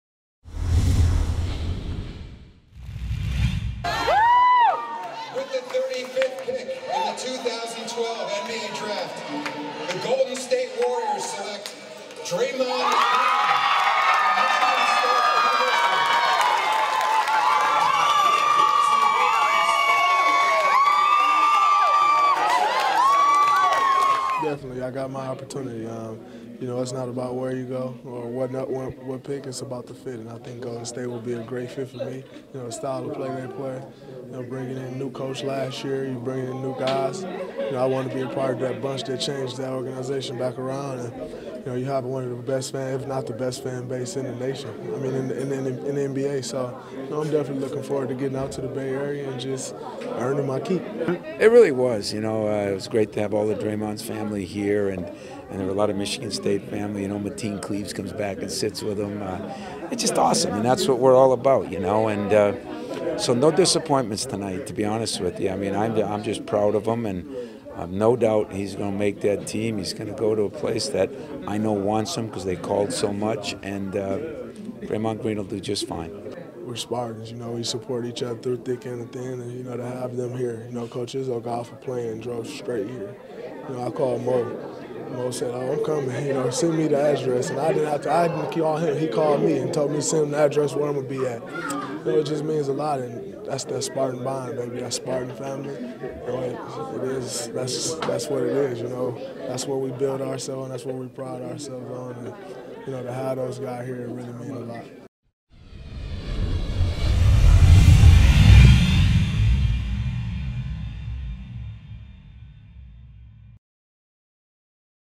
Draymond Green Selected in 2012 NBA Draft
Michigan State Forward Draymond Green was selected 35th overall by the Golden State Warriors in the 2012 NBA Draft.